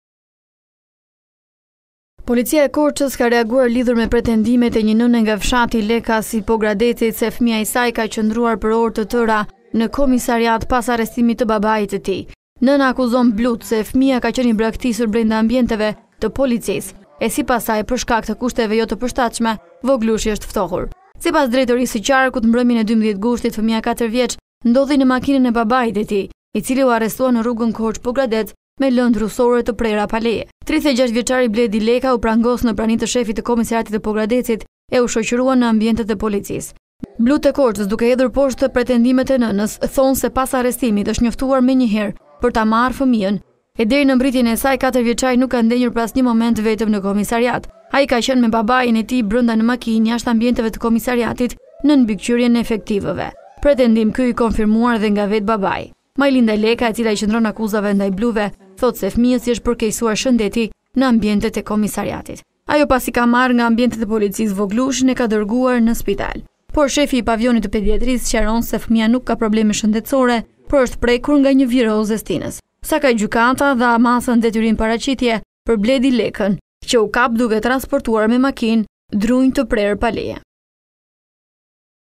A1 Report - Nëna:Më mbajtën djalin 4-vjeç në polici, blutë: E lamë me të atin
POGRADEC - Policia e Korçës ka reaguar zyrtarisht lidhur me pretendimet e një nëne nga fshati Lekas i Pogradecit se fëmija i saj ka qëndruar për orë të tëra në komisariat.

Sipas policisë mbrëmjen e 12 gushtit fëmija 4 vjeç ndodhej në makinën e babait të tij, i cili u arrestua në rrugën Korçë –Pogradec me lëndë drusore të prera pa leje.

36 vjeçari, Bledi Leka u arrestua në prani të shefit të komisariatit të Pogradecit dhe u shoqërua në ambientet e policisë.

Blutë e Korçës sqarojnë se menjëherë pas arrestimit është njoftuar nëna e fëmijës për ta marrë atë dhe deri në mbërritjen e saj 4-vjeçari nuk ka ndenjur për asnjë moment të vetëm në komisariat.

Policia thotë se i mituri ka qëndruar me babain e tij brenda në makinë, jashtë ambienteve të komisariatit nën mbikëqyrjen e efektivëve. Pretendim ky i konfirmuar edhe nga vetë babai.

Majlinda Leka, e cila i qëndron akuzave ndaj bluve, thotë se fëmija është sëmurur në ambientet e komisariatit.

Ajo pasi e ka marrë nga ambientet e policisë, e ka dërguar fëmijën në spital. Por shefi i Pavionit të Pediatrisë sqaron se fëmija nuk ka probleme shëndetësore, por është prekur nga një virozë e stinës.

Sakaq Gjykata dha masën “Detyrim Paraqitje” për Bledi Leka, që u kap duke transportuar me makinë drunj të prerë e pa leje.